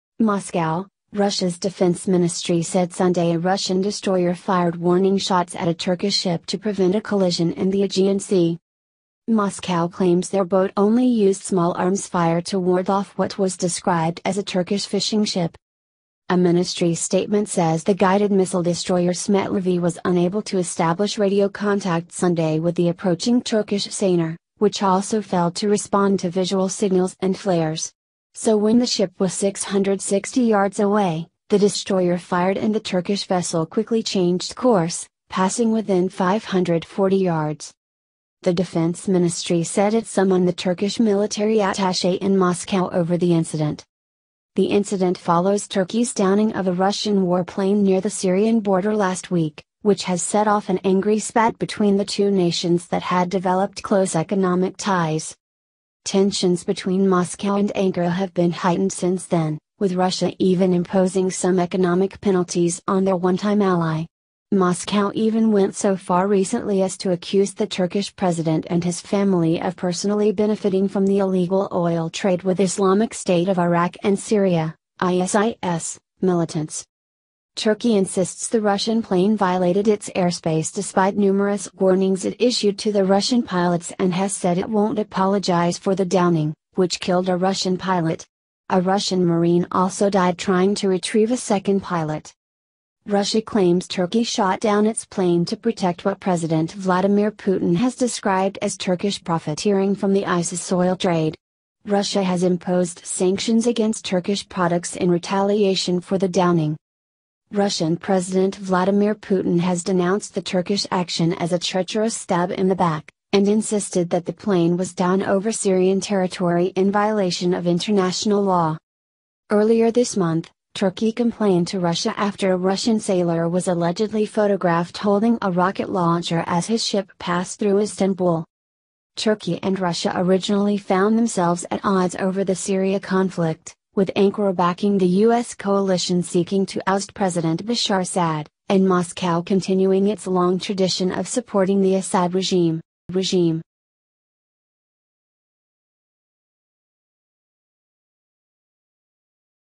Russian destroyer fires warning shots at Turkish fishing boat
MOSCOW - Russia's Defense Ministry said Sunday a Russian destroyer fired warning shots at a Turkish ship to prevent a collision in the Aegean Sea.

Moscow claims their boat only used small arms fire to ward off what was described as a Turkish fishing ship.

A ministry statement says the guided missile destroyer Smetlivy was unable to establish radio contact Sunday with the approaching Turkish seiner, which also failed to respond to visual signals and flares. So when the ship was 660 yards away, the destroyer fired and the Turkish vessel quickly changed course, passing within 540 yards.

The Defense Ministry said it summoned the Turkish military attache in Moscow over the incident.

The incident follows Turkey's downing of a Russian warplane near the Syrian border last week, which has set off an angry spat between the two nations that had developed close economic ties.

Tensions between Moscow and Ankara have been heightened since then, with Russia even imposing some economic penalties on their one-time ally. Moscow even went so far recently as to accuse the Turkish president and his family of personally benefiting from the illegal oil trade with Islamic State of Iraq and Syria (ISIS) militants.

Turkey insists the Russian plane violated its airspace despite numerous warnings it issued to the Russian pilots and has said it won't apologize for the downing, which killed a Russian pilot. A Russian marine also died trying to retrieve a second pilot.

Russia claims Turkey shot down its plane to protect what President Vladimir Putin has described as Turkish profiteering from the ISIS oil trade. Russia has imposed sanctions against Turkish products in retaliation for the downing.

Russian President Vladimir Putin has denounced the Turkish action as a "treacherous stab in the back," and insisted that the plane was downed over Syrian territory in violation of international law.

Russia fires warning shot at Turkey boat in Aegean
BBC News - ‎1 hour ago‎
Russia says one of its warships fired warning shots at a Turkish fishing vessel in the Aegean Sea to avoid a collision. A Russian defence ministry statement said the Turkish vessel approached to 600m (1,800ft) before turning away in response to Russian ...

Russian warship fires warning shots at Turkish trawler to avoid collision
The Guardian - ‎34 minutes ago‎
A Russian warship has been forced to fire warning shots at a Turkish fishing vessel to avoid the two boats colliding, the defence ministry in Moscow has confirmed. Russian state media reported that the Smetlivy, a military vessel, fired the hots across ...

Russian destroyer fires warning shots at Turkish fishing boat on collision ...
RT - ‎1 hour ago‎
The crew of the Kashin-class guided missile destroyer Smetlivy spotted the Turkish fishing trawler some 1,000 meters from the Russian warship early on Sunday morning, the ministry said. The ship was sailing towards the destroyer and ignored calls for ...

Russian Ship in Aegean Sea Fires Warning Shots at Turkish Vessel
Wall Street Journal - ‎25 minutes ago‎
MOSCOW—Russia summoned Turkey's military attache in Moscow after a Russian patrol ship fired warning shots at a Turkish vessel in the Aegean Sea, the Russian military said Sunday. According to the statement, the warship Smetlivy fired ... “Despite ...

BREAKING: Russian warship FIRES at Turkish vessel in the Aegean in stark warning
Express.co.uk - ‎39 minutes ago‎
They said in a statement that the Turkish boat had approached to 600m. The Russian ship had been unable to make radio contact and the Turkish vessel did not respond to flares or visual signals. After the Russian "guard ship" Smetlivy fired warning ...

Russia fires warning shots at Turkish ship in Aegean Sea
CBS News - ‎42 minutes ago‎
MOSCOW - Russia's Defense Ministry said Sunday a Russian destroyer fired warning shots at a Turkish ship to prevent a collision in the Aegean Sea. Moscow claims their boat only used small arms fire to ward off what was described as a Turkish fishing ship.
Russia Warship Fires Warning to Stop Collision With Turkish Boat
Bloomberg - ‎51 minutes ago‎
A Russian warship fired a warning shot to prevent a collision with a Turkish fishing vessel in the Aegean sea, further straining ties between the two nations after last month's downing of a Russian bomber near the Syrian border. Russia's Smetliviy ...

Russian warship fires 'warning shots' at Turkish vessel in Aegean Sea clash
The Independent - ‎52 minutes ago‎
A Russian destroyer has fired warning shots at a Turkish vessel in the latest clash between the two countries in the Aegean Sea, officials have said. The Russian Defence Ministry told reporters it had summoned the Turkish military attache over the ...

Russian Federation fires Turkey boat warning shot
ChronicleDaily.com (blog) - ‎28 minutes ago‎
"Russian Deputy Defense Minister Anatoly Antonov has summoned the military attaché of the